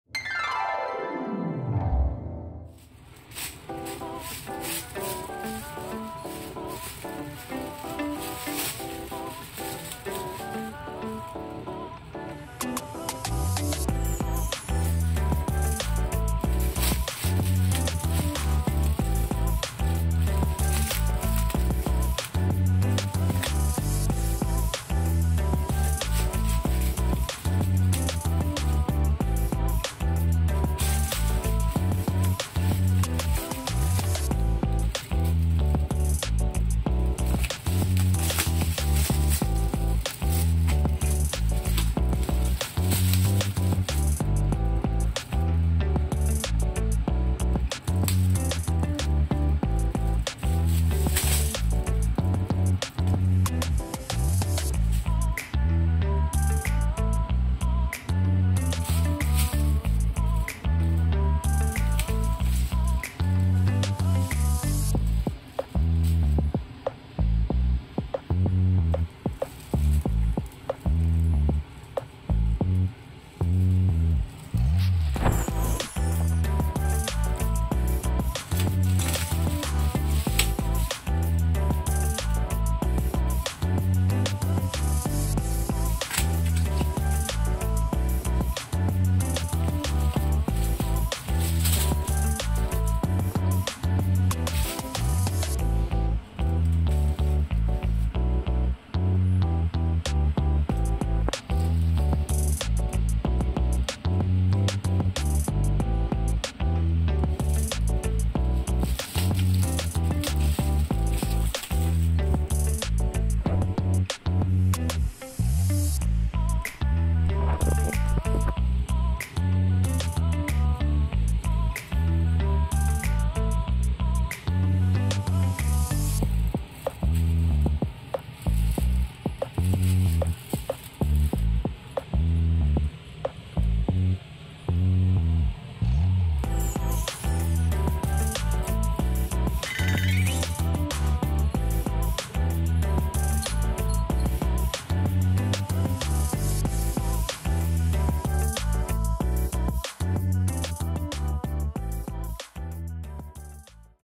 キッチンでガサゴソ。サステナブル猫⁉︎ Cats Mischief in the Kitchen【アメリカンショートヘア】子猫
アメショのきょうだい猫　 ルナ(♀)とメロ(♂)。
キッチンでゴミ袋をイタズラする猫の目的とは。